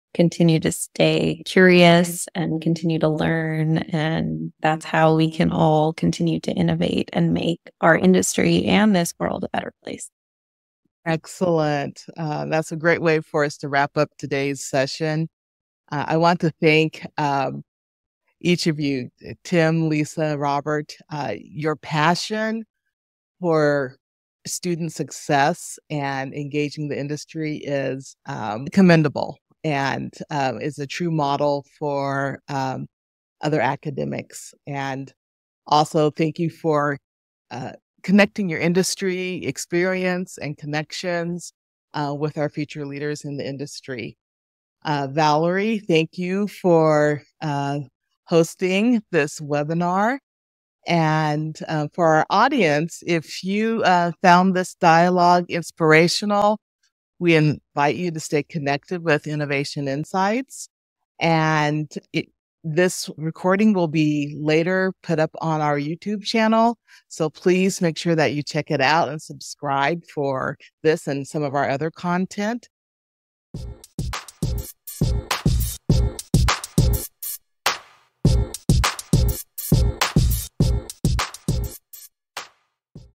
Stay Curious: The Path to Innovation | INNOVATION Insights LIVE
In this episode of INNOVATION Insights LIVE, Tim, Lisa, and Robert share their commendable passion for student success and industry engagement. Dr. Valerie  Salter, hosts the webinar, emphasizing the importance of continuous learning and innovation for a better future. The recording will be available on our YouTube channel for those who found the dialogue inspirational.

Discover exclusive gift items, empowering gift cards, transformative seminars and workshops, and free creative coloring pages.